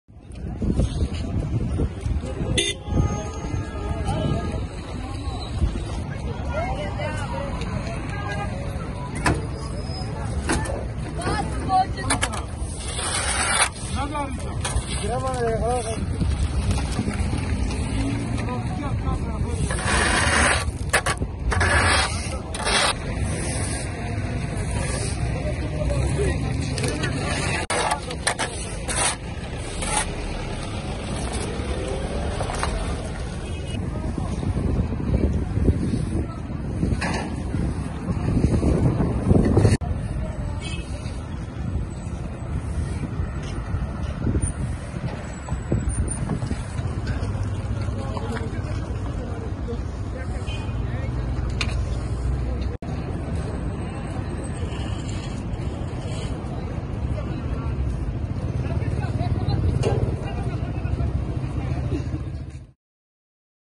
Men clean up street after deadly Kabul rocket attack | AFP
Workers clean a street after a series of rockets struck Kabul, killing one person and wounding two, according to the interior ministry. It was the second such attack to rock the Afghan capital in less than a month.